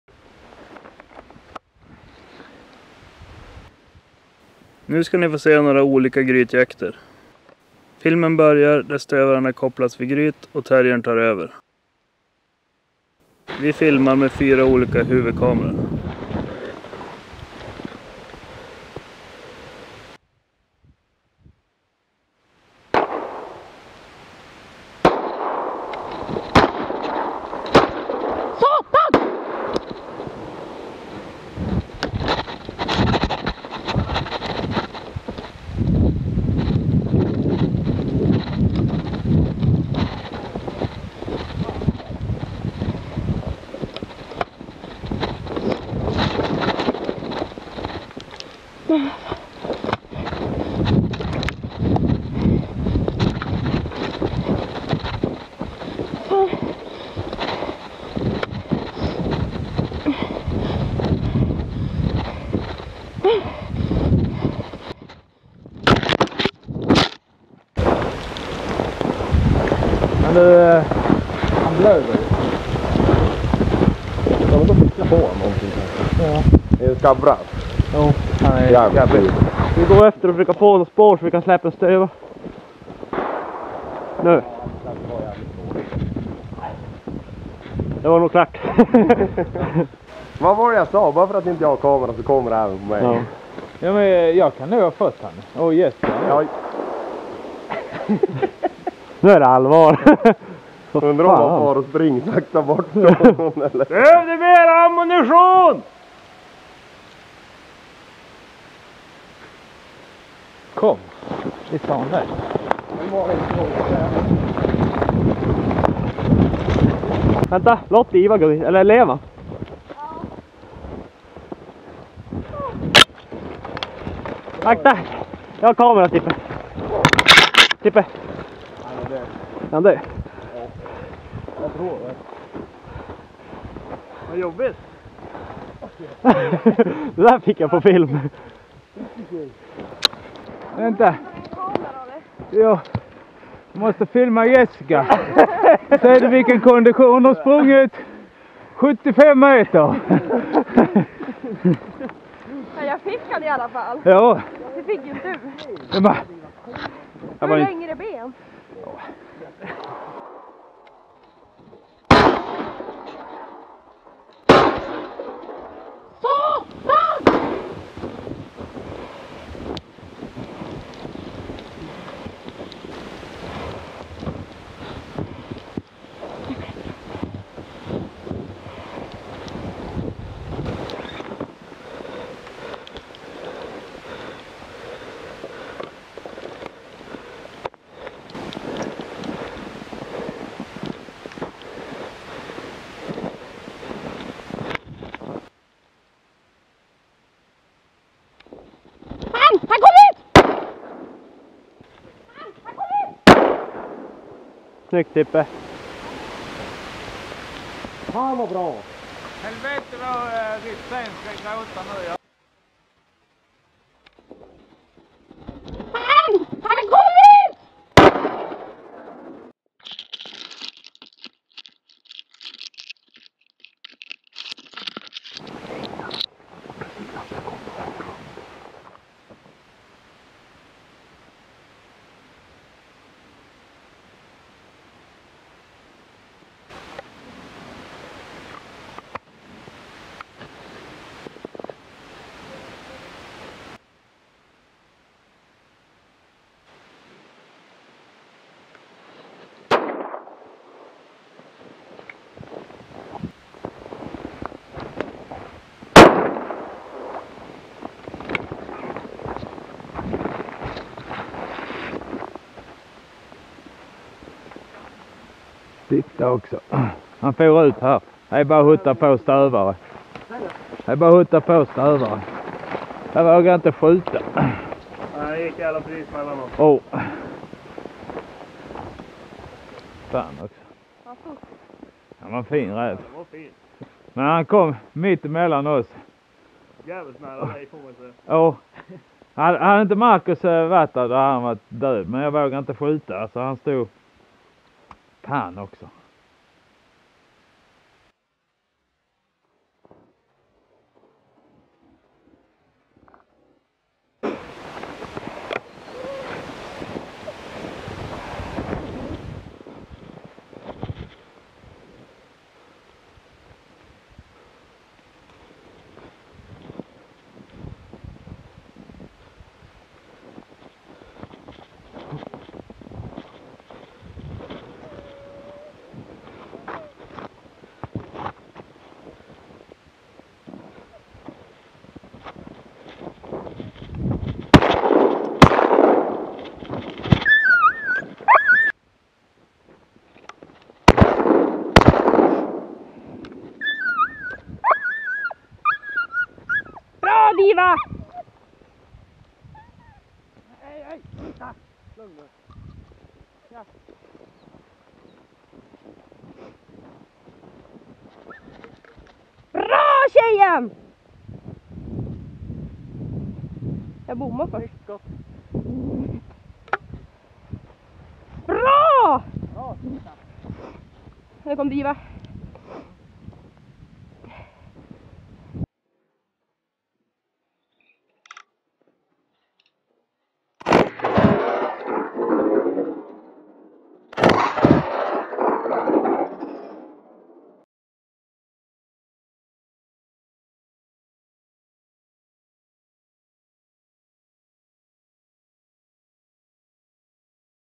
Grytjakt på räv - Fox hunting with terrier - Ketun metsästys - Fuchsjagd - Охота на Лиса
English sub

Grytjakt med fox terrier
Följ för att se kommande filmer. Instagram: markusjonort